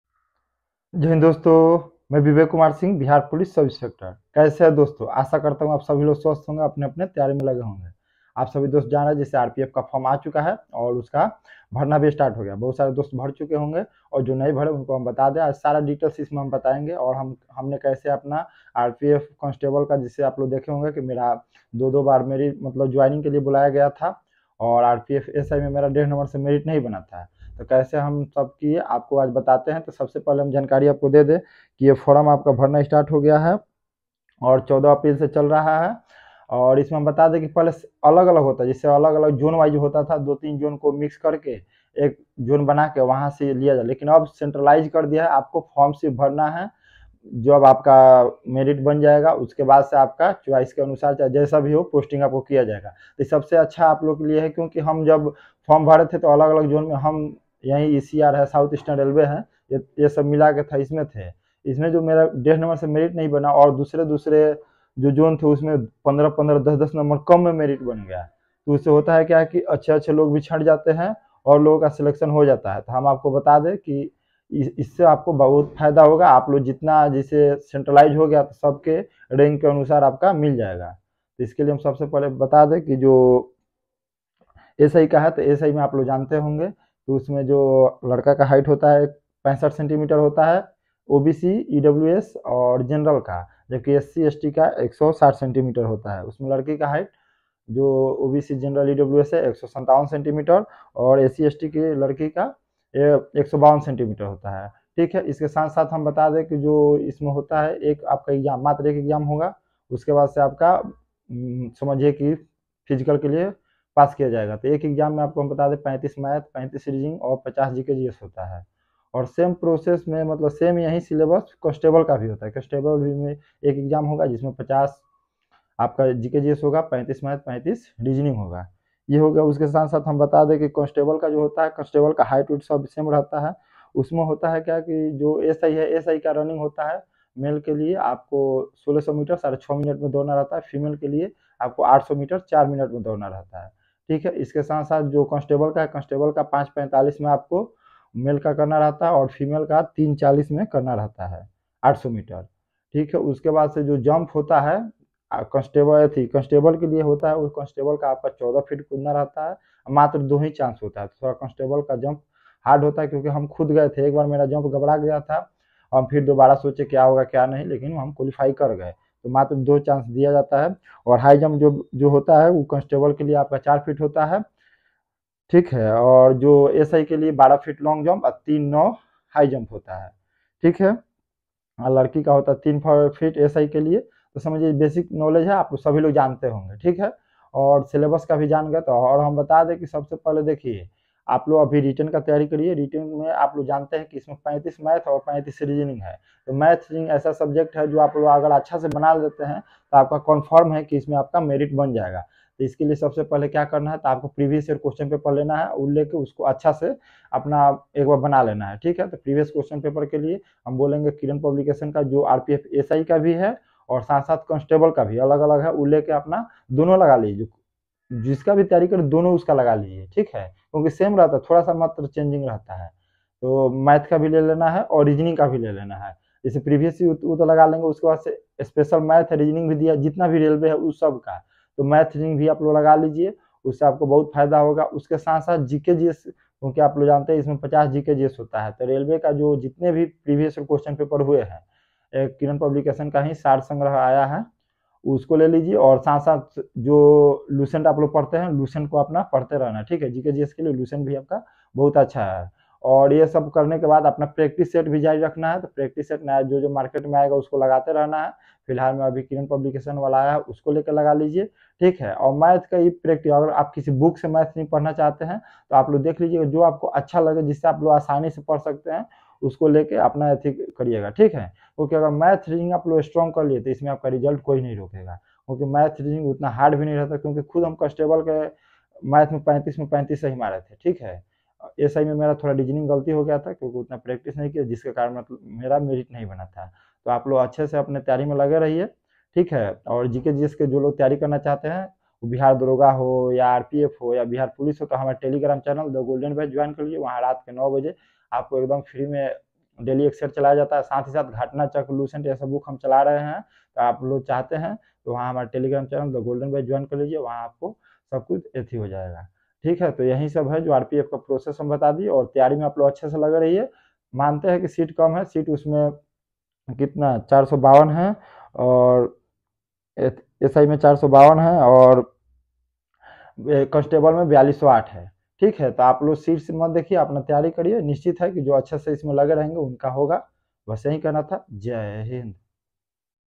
RPF SI & CONSTABLE NEW VACANCY 2024 | ELIGIBILITY | BOOK LIST | PHYSICAL EFFICIENCY
Kismat badalti dekhi ❤️⭐⭐ SI VIVEK KUMAR SINGH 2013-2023 Struggle ⭐⭐